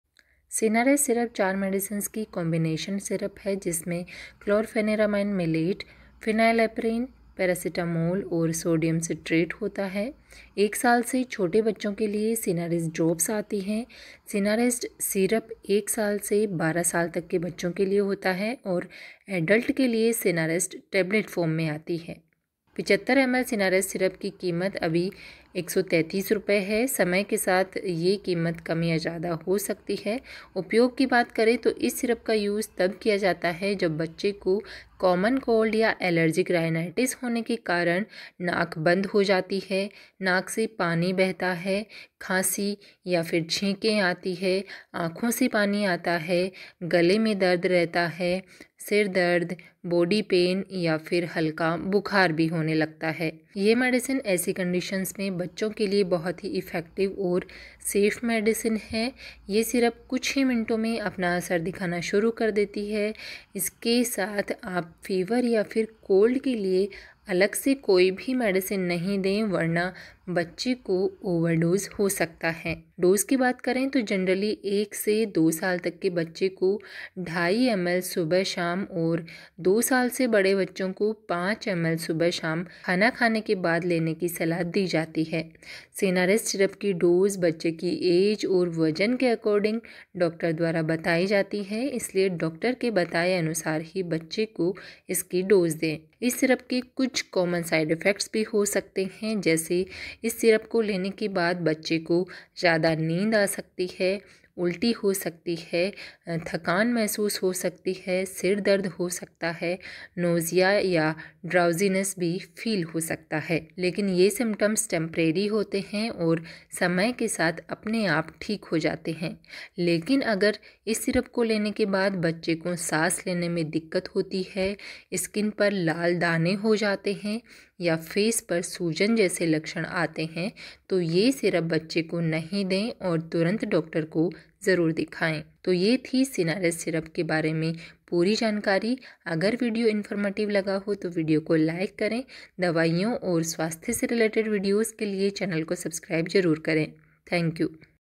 Sinarest syrup price uses doses side effects बच्चों के लिए सर्दी, खांसी और बुखार की सबसे अच्छी दवा
Sinarest syrup is used to treat cold, cough , fever and allergic symptoms in children. This is the informational video about sinarest syrup . Watch till the end for full information.

This is the best syrup for babies.
Note:- Self medication can be hazardous so never take any medicine without prescription of medical practitioner.